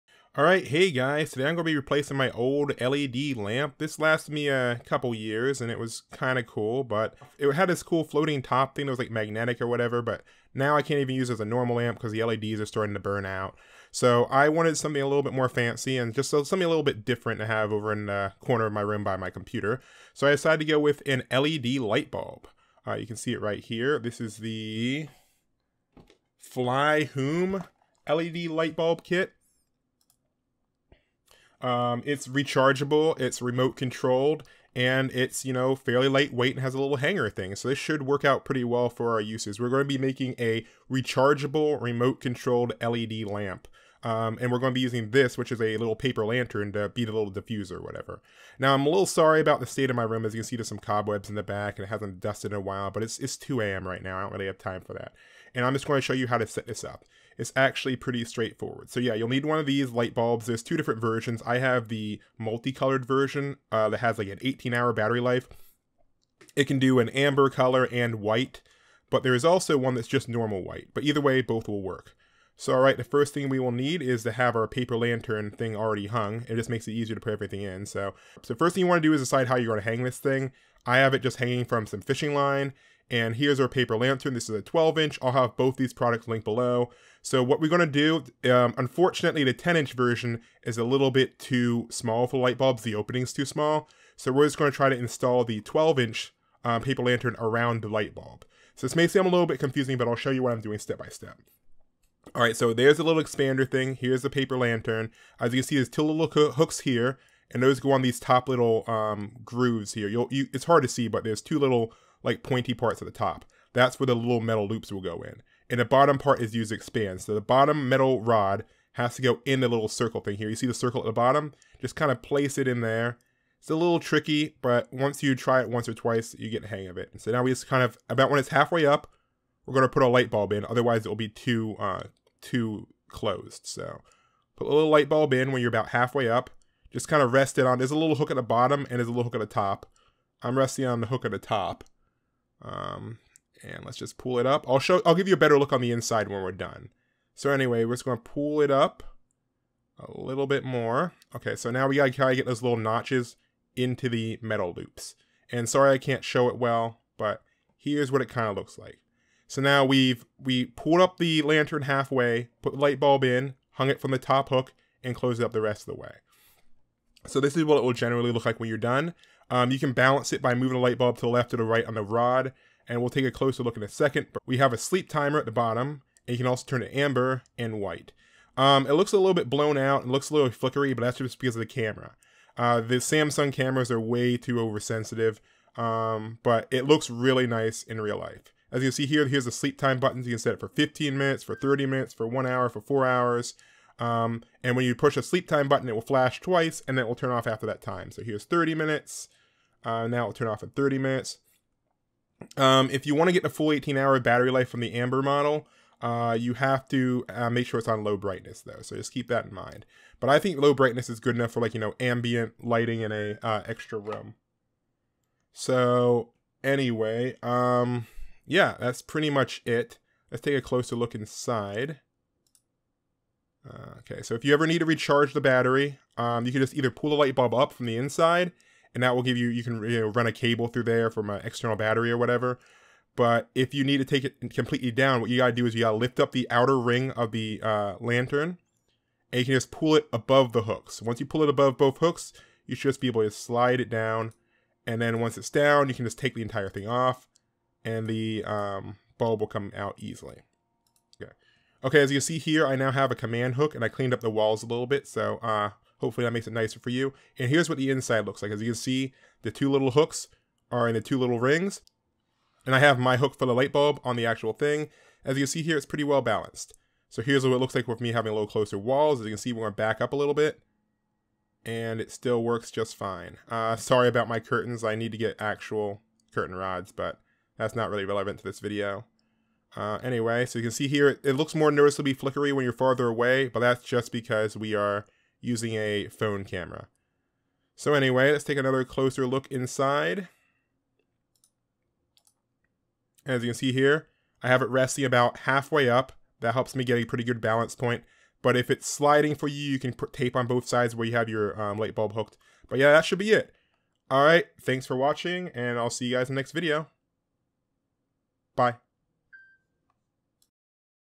(Mini Project) Rechargeable Remote Controlled Paper Lantern!
Today we add a Rechargeable Led lightBulb to a paper lantern!
Sure, there are bulbs made for these things, but they're often not remote control+rechargeable!

Paper Lanterns (12" used):